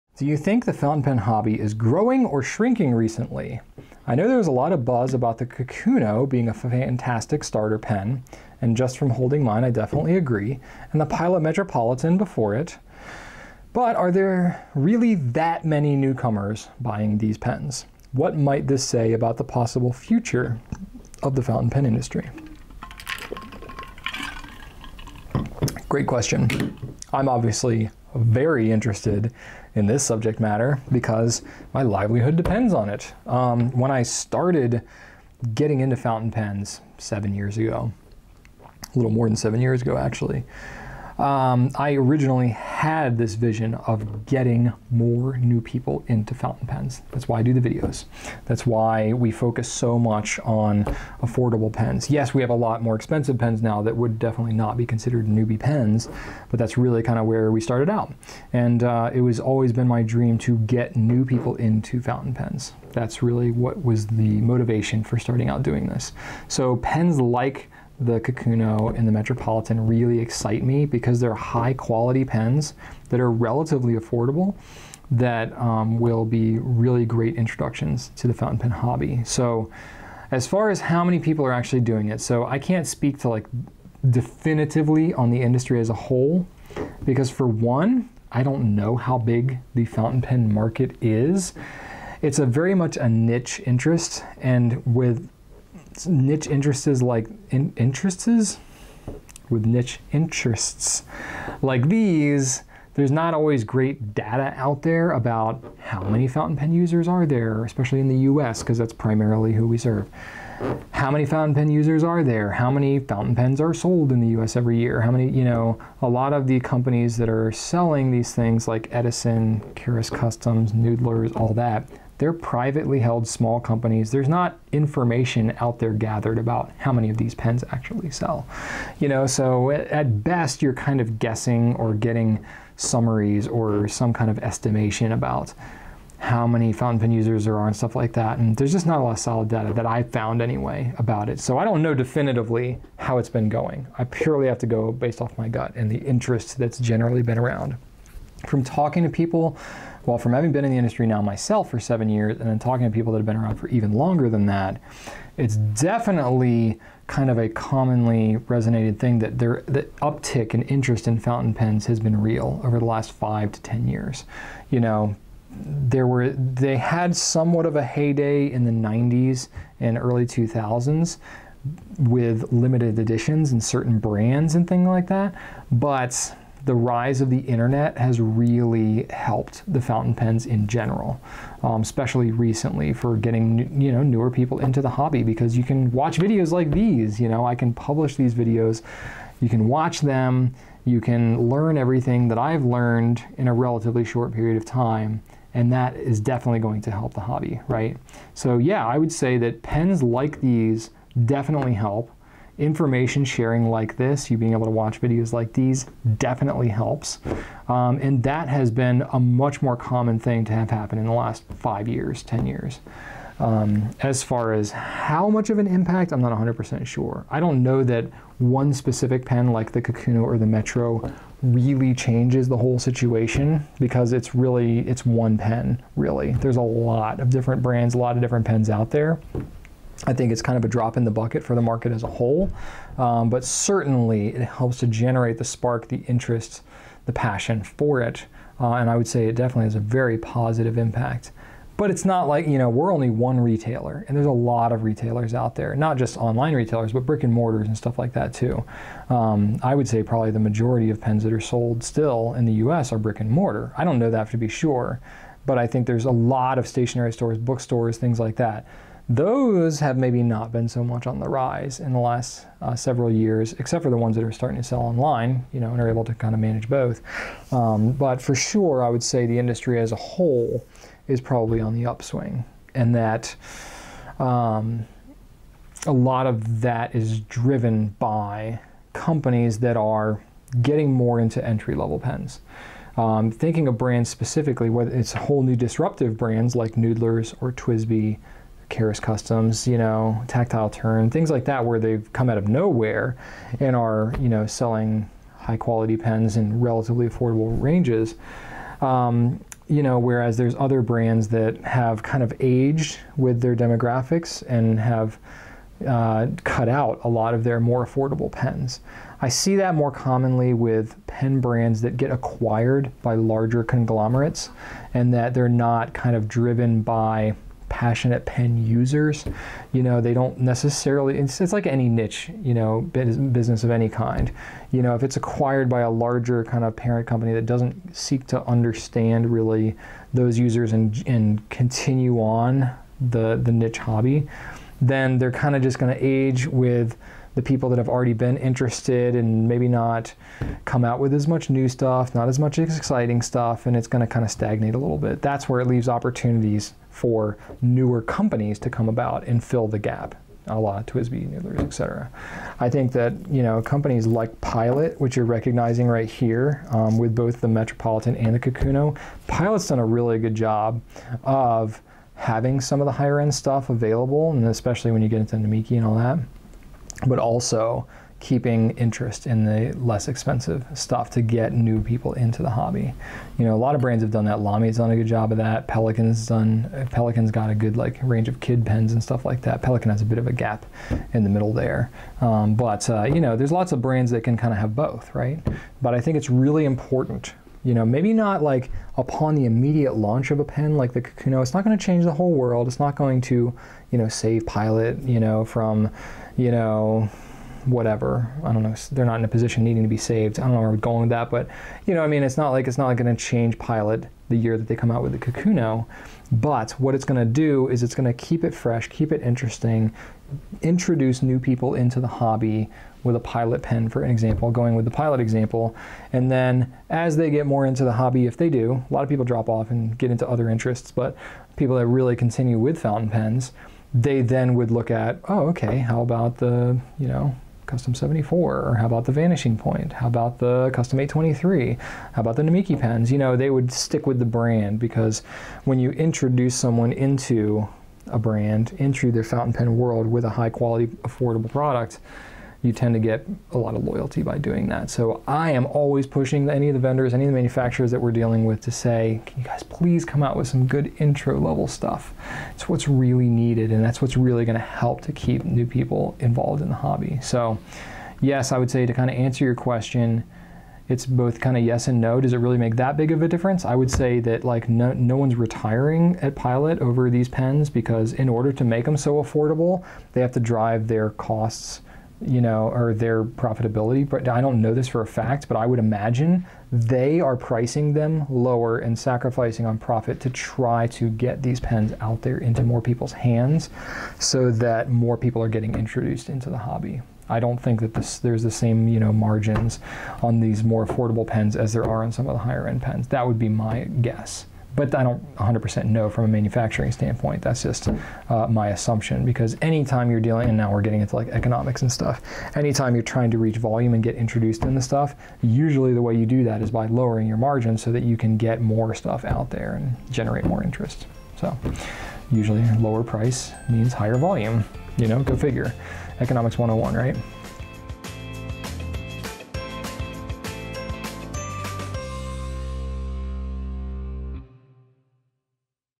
Is The Fountain Pen Business Growing Or Shrinking? - Q&A Slices
Do you think the fountain pen hobby is growing or shrinking recently? I know there was a lot of buzz about the Kakuno being a fantastic starter pen (and just from holding mine, I definitely agree), and the Pilot Metropolitan before it, but are there really that many newcomers buying these pens? What might this say about the possible future of the fountain pen industry?

Globally it is growing, yes, especially in Asia
hard to say in the US
GouletPens has definitely been growing, I think online sales have been growing, I'm not sure about B&M's
the Metro and Kakuno are definitely attracting newbies, same with TWSBI and Noodler's
the fountain pen industry is certainly evolving, and a lot of companies are starting to recognize that getting quality entry-level pens are important for long-term growth
those that aren't introducing new entry-level products are going to have a tougher time staying relevant, and will rely more on their brand name than anything
exceptions would be luxury brands, but even they are looking to introduce (relatively) entry-level pens (Visconti, Montegrappa, Aurora, etc)
I personally am very optimistic, because I think there will be a resurgence of analog writing in response to an increasingly technical world
the companies that recognize this and move in this direction will have the most advantage